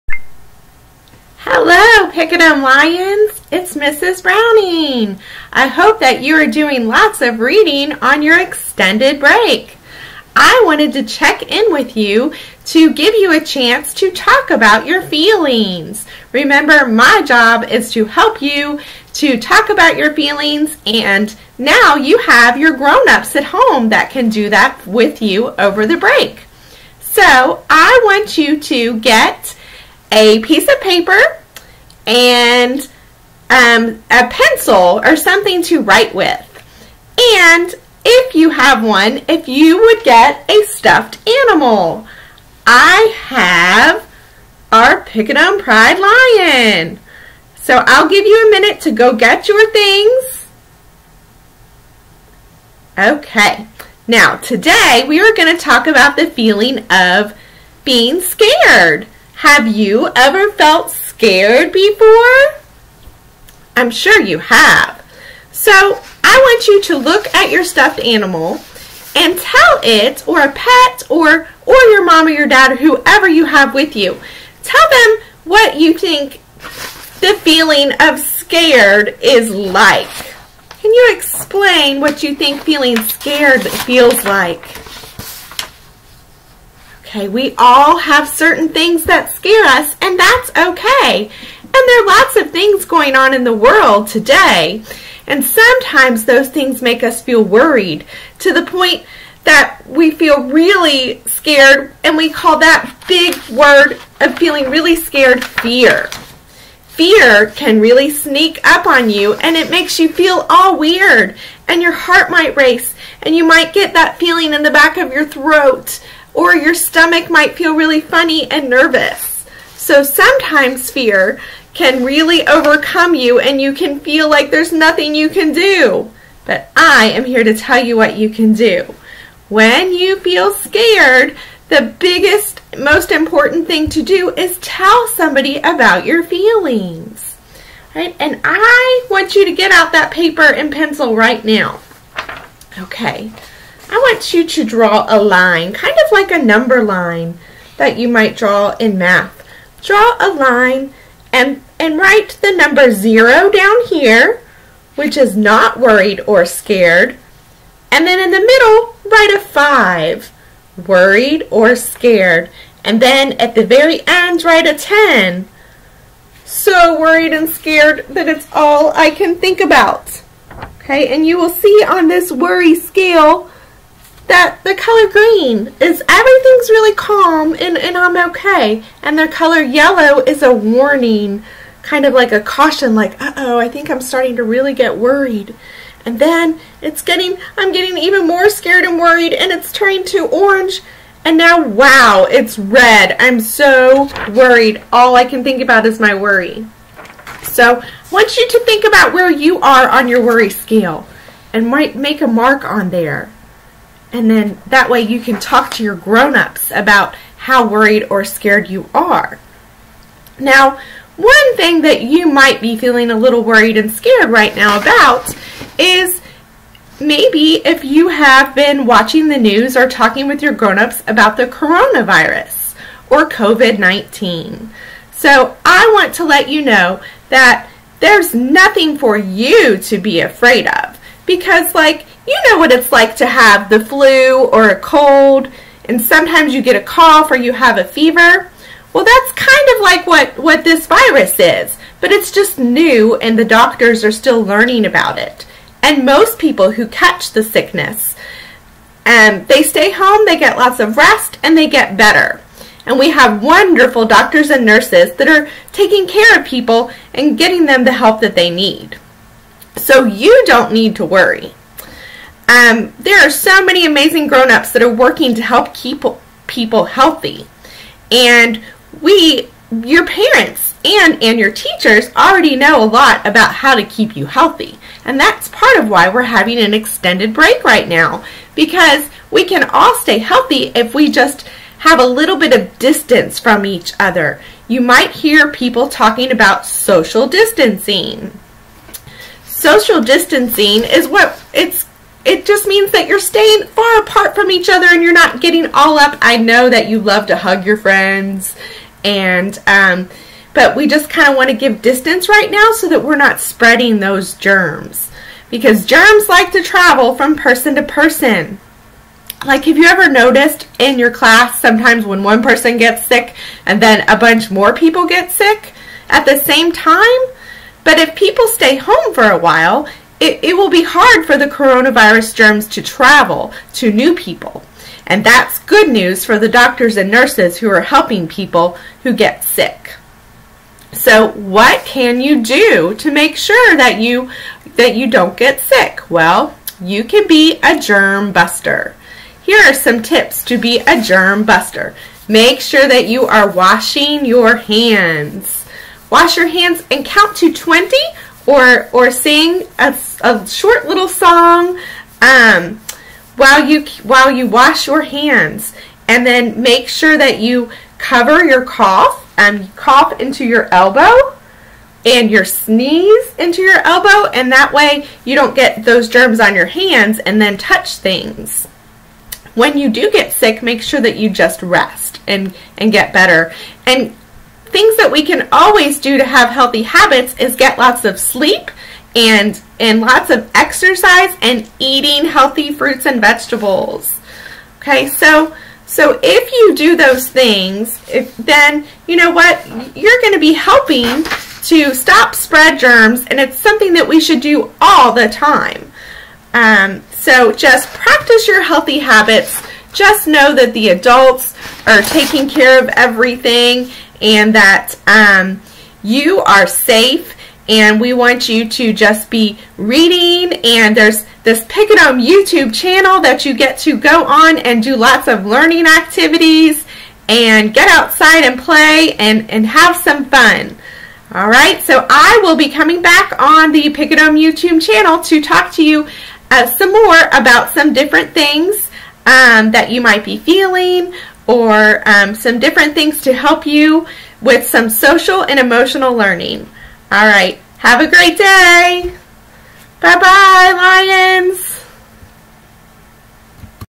Be a Germbuster!
Great for kids to explain the extended leave from school. Information from the CDC site was used.